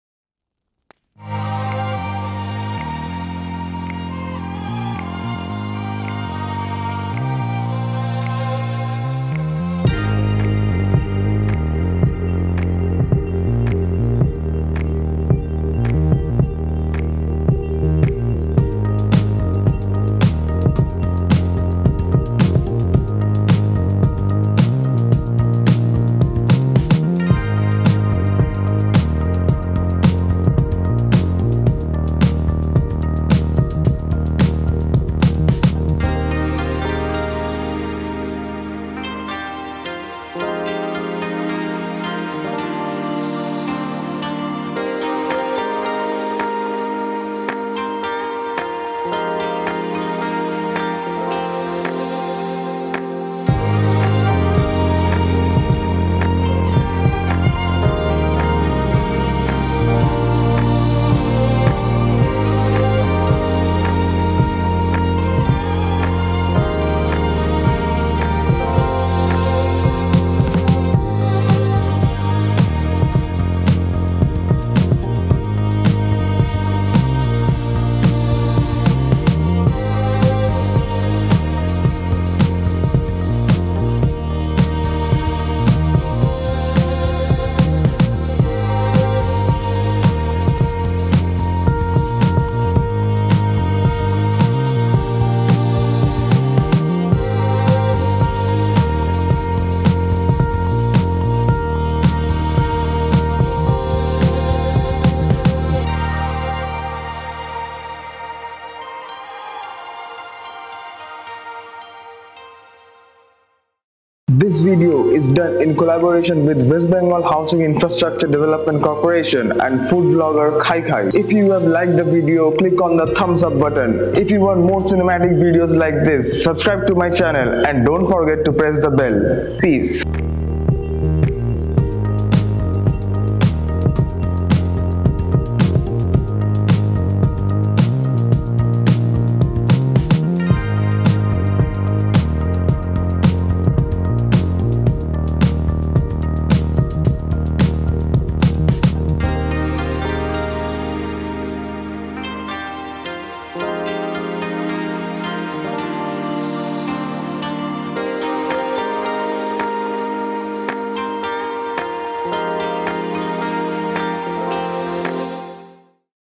BISWA BANGLA GATE - Hanging Restaurant | Cinematic Aerial Video | Newtown | The Pride of Kolkata
This cinematic video is done in collaboration with HIDCO and food vlogger Khai Khai. Behold the magic, witness the spectacle that is the Biswa Bangla Gate. An architectural marvel that is a testament to the efficient and quality execution of the concept.